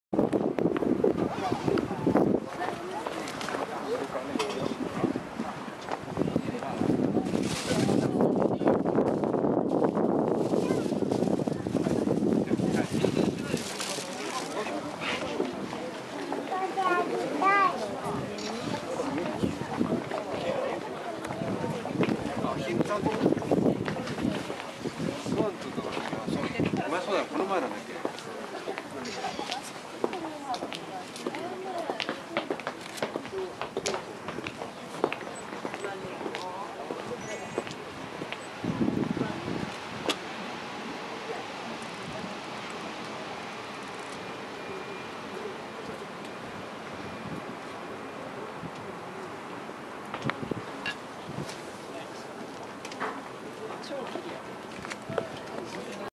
卡霧的函館夜景
蒙著一層霧的函館夜景看起來有著淡淡的哀愁，且山上氣溫驟降，原本覺得還挺涼爽的，不一會兒就立刻整個人從內開始發寒，且還下著毛毛雨~XD
希望還有機會去北海道的話可以看到清晰的夜景。